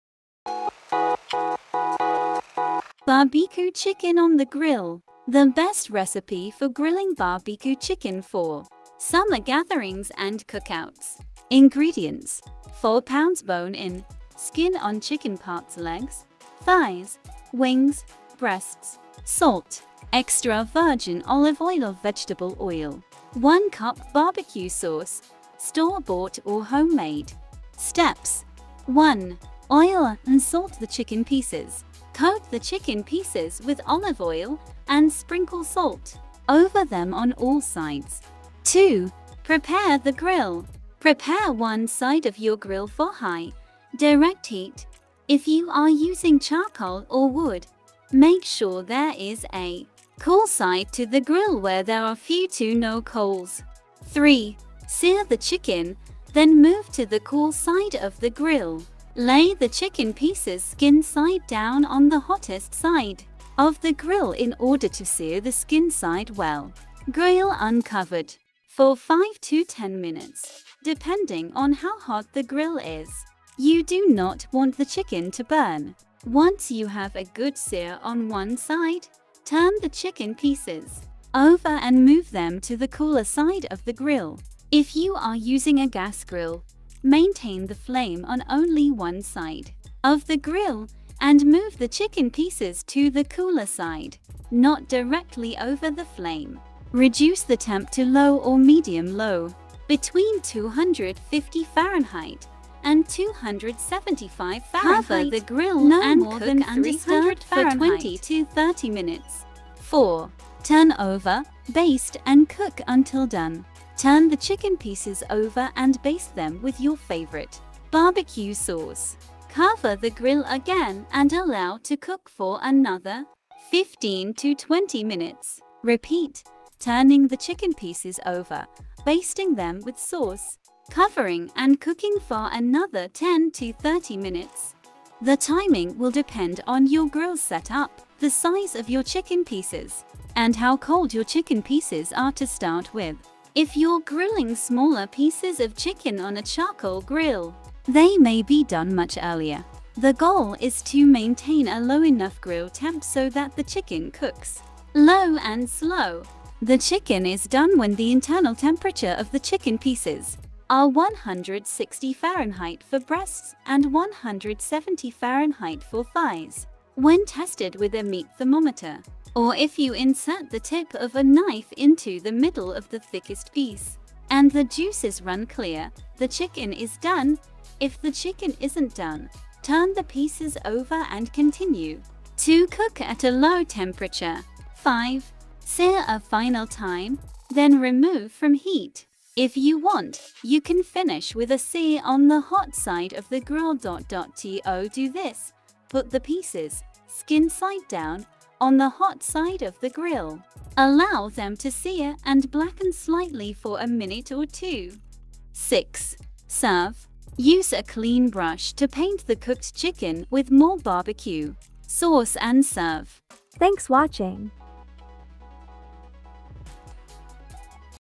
BBQ Chicken on the Grill
𝐈𝐧𝐠𝐫𝐞𝐝𝐢𝐞𝐧𝐭𝐬 :

• 4 pounds bone-in, skin-on chicken parts (legs, thighs, wings, breasts)
• Salt
• Extra virgin olive oil or vegetable oil
• 1 cup barbecue sauce, store-bought or homemade